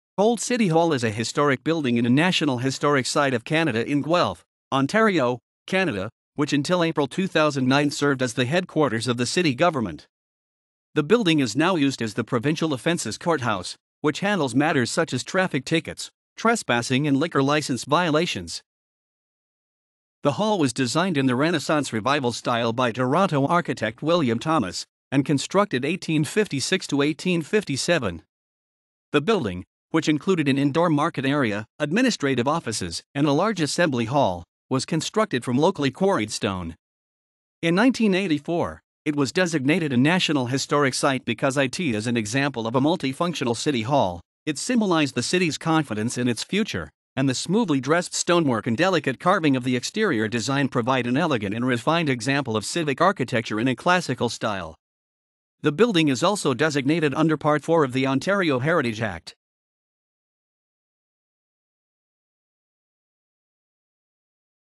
Old City Hall (Guelph)
Old City Hall is a historic building and a National Historic Site of Canada in Guelph, Ontario, Canada, which until April 2009 served as the headquarters of the city government.  The building is now used as the Provincial Offences Courthouse, which handles matters such as traffic tickets, trespassing and liquor license violations.